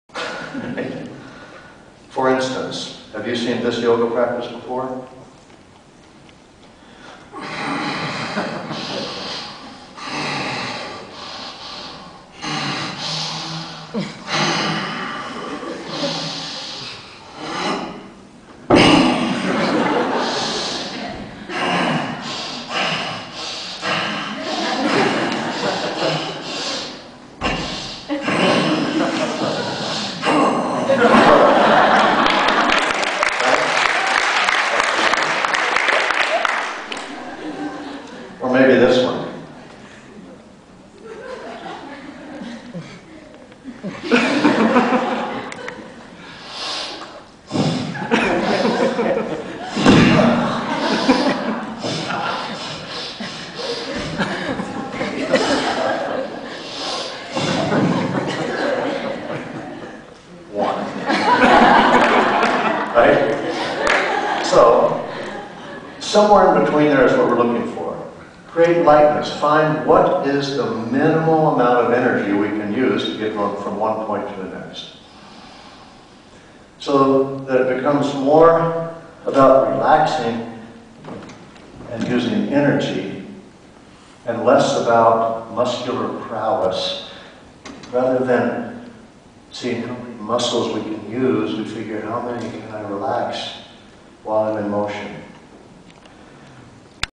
David swenson!-sun-salutation demo, 3 types of practitioners.
David Swenson,  the human being behind the famous name. one of the original group of Ashtanga Practitioners and teachers, 11 men and women that made the Ashtanga spread all over thye world. 4 short videos, the human being behind the famous name. a great teacher and a great personal friend.